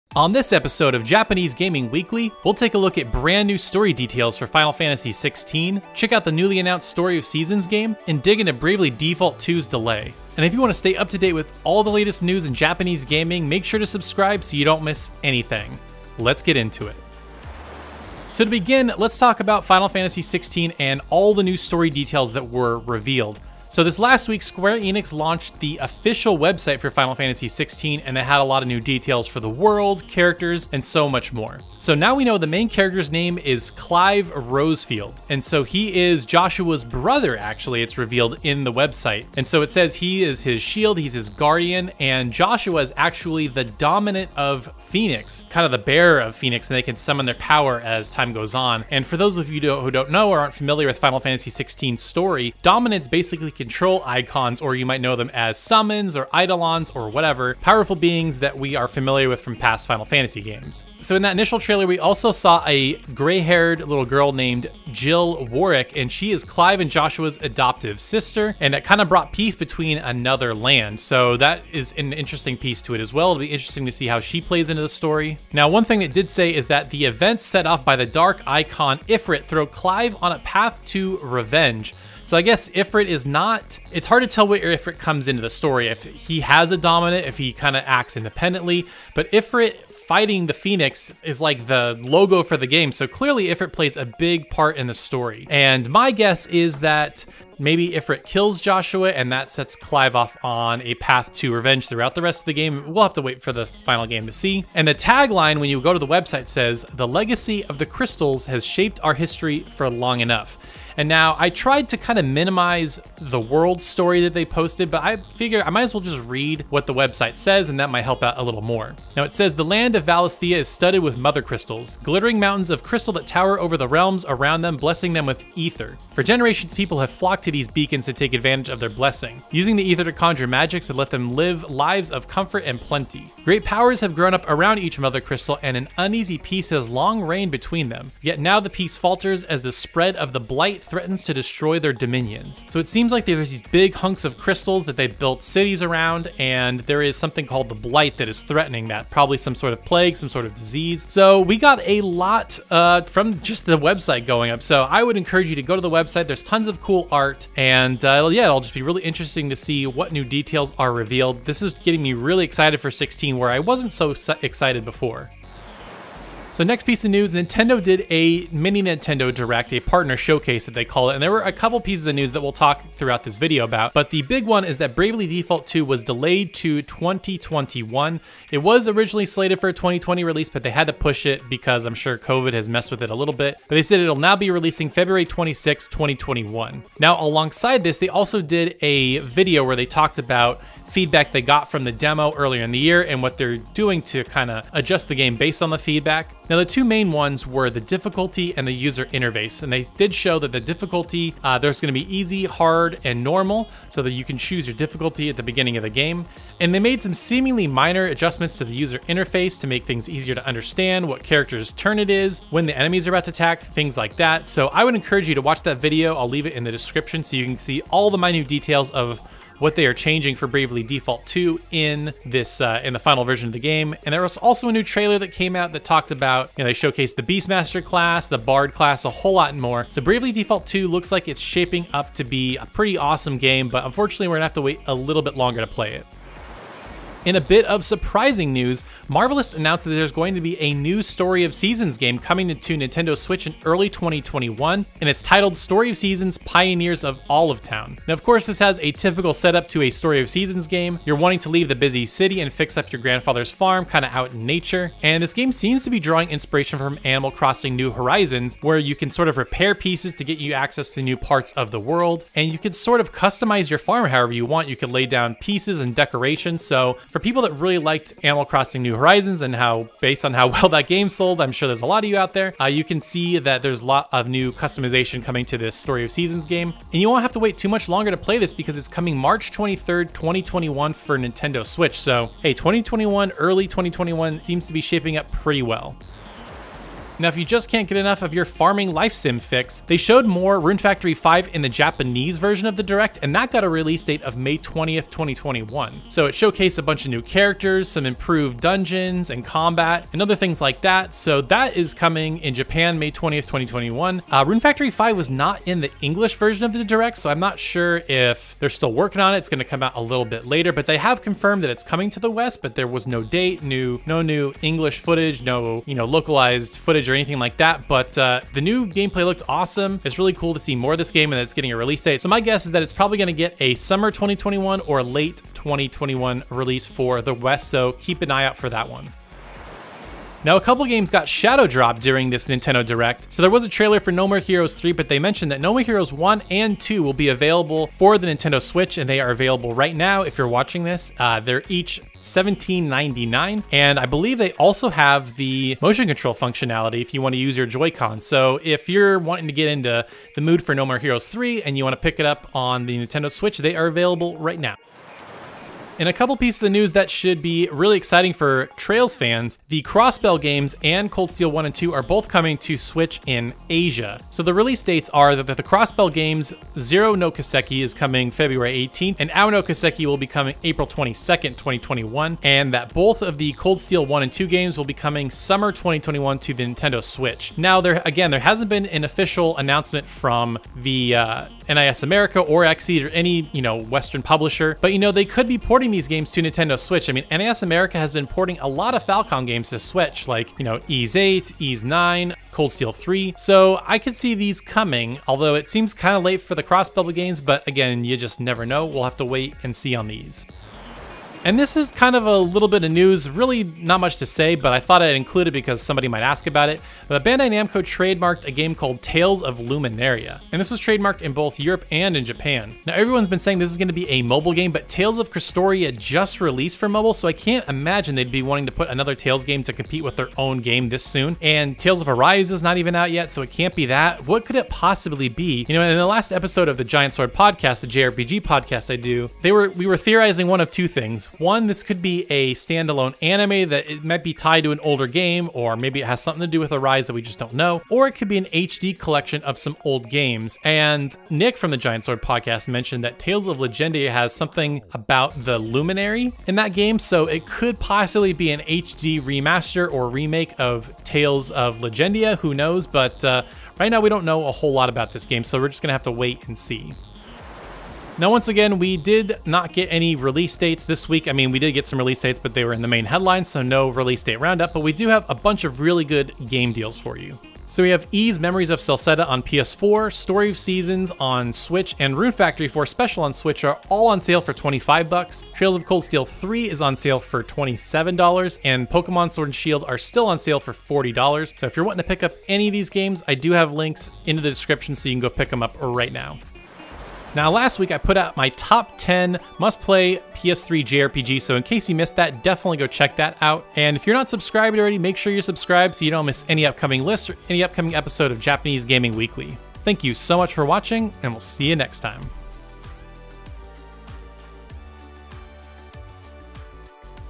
New Final Fantasy XVI Story Details REVEALED! | Japanese Gaming Weekly
On this episode of Japanese Gaming Weekly: we look at the new story details revealed for Final Fantasy XVI, check out the newly announced Story of Seasons game and dig into Bravely default II's delay. All this and more!

Game Deals:
Ys: Memories of Celceta (PS4)
Trails of Cold Steel III (PS4)
Story of Seasons: Friends of Mineral Town
Rune Factory 4: Special: (Deal expired)
Pokémon Sword
Pokémon Shield

Timestamps:
Intro 0:00
Headlines 0:18
Game Deals 7:47

🎙️ My Gear:
My Mic
My Capture Card
My Streaming Webcam
My Filming Camera
My Gaming & Editing Laptop